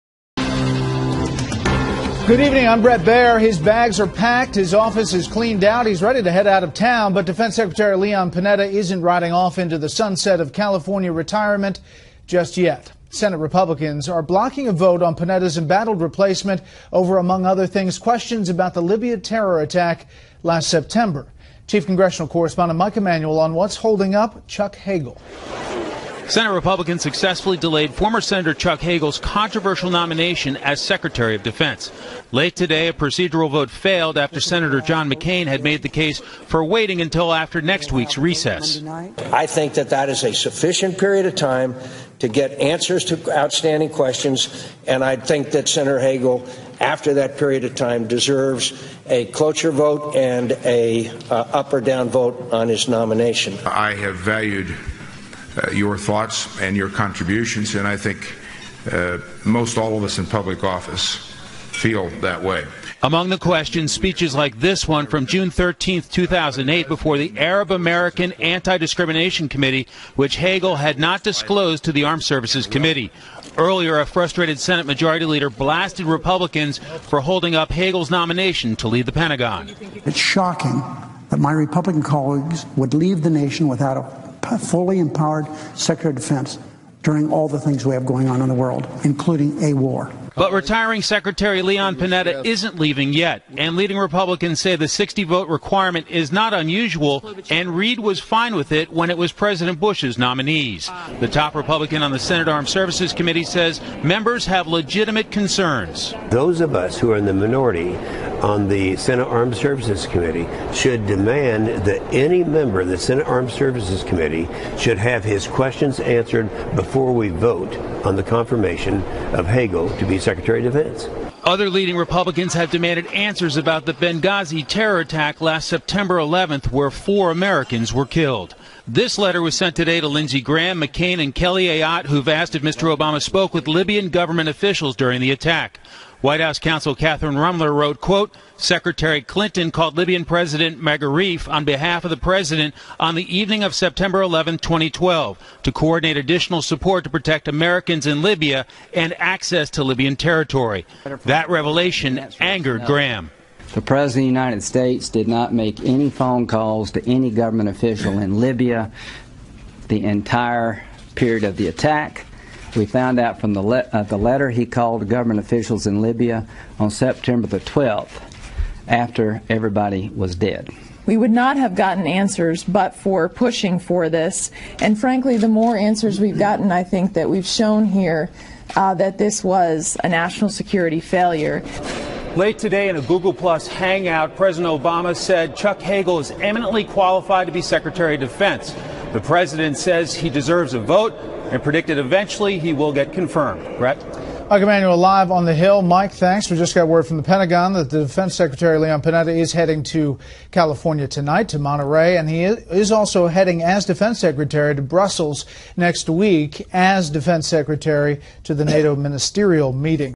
Senate Republicans Successfully Delay Hagel Nomination
What is holding up confirmation process?